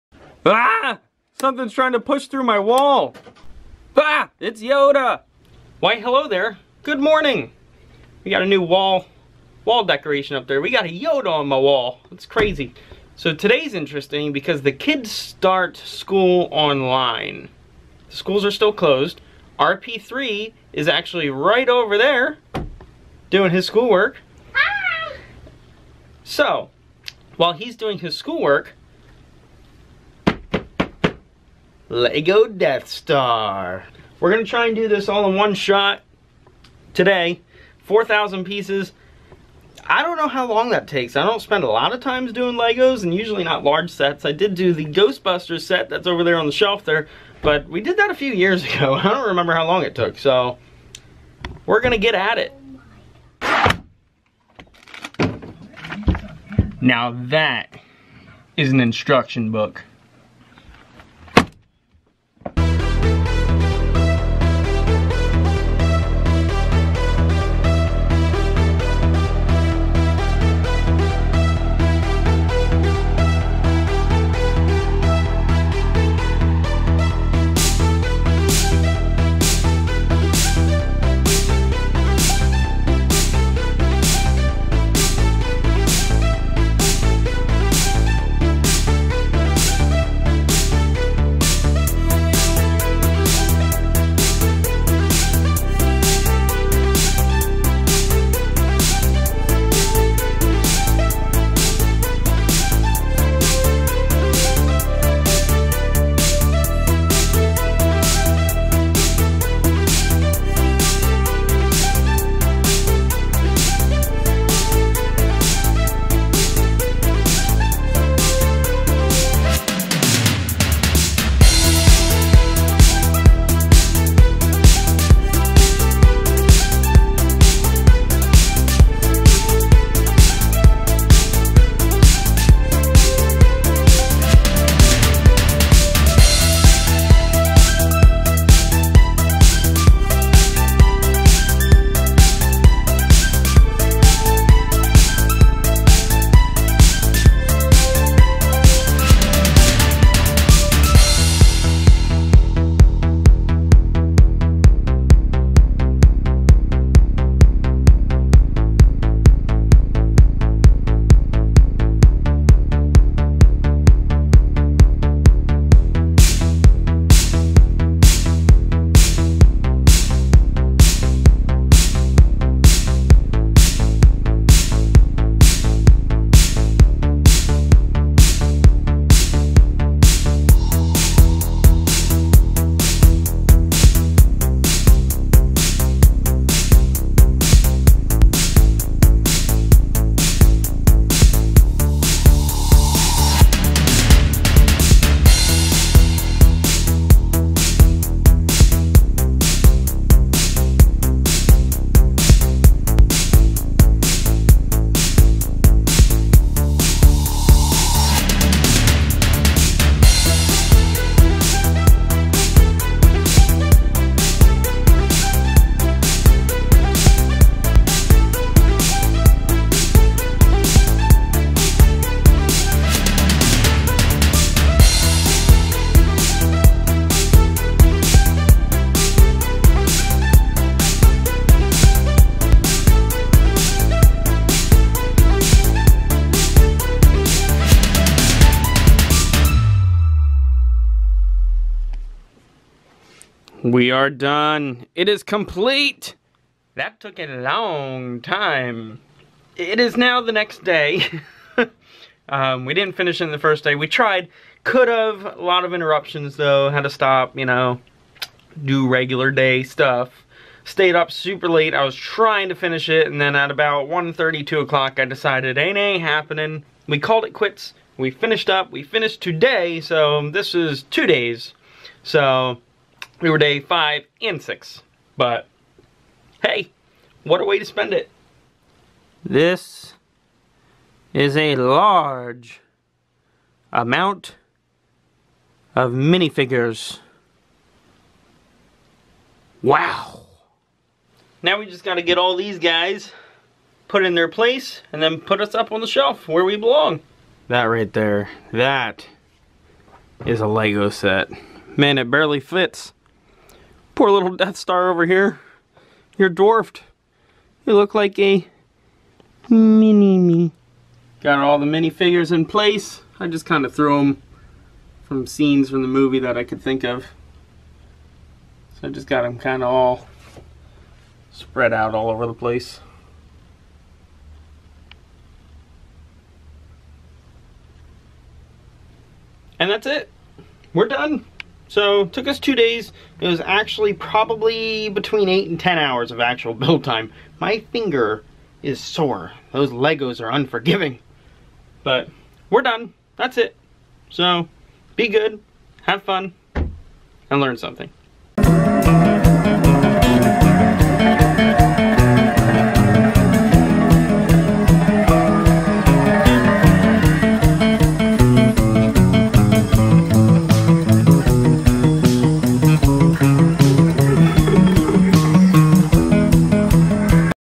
Lego Death Star Full Build | Time Lapse | Laid off day 5 and 6 Vlog
tackling the Lego death star. Over 4000 pieces it is in the top ten largest Lego sets. Time lapse video of the complete build

Please leave comments/suggestions on what content you would like to see! Thank you for your support.

RSP58
RSP58Gaming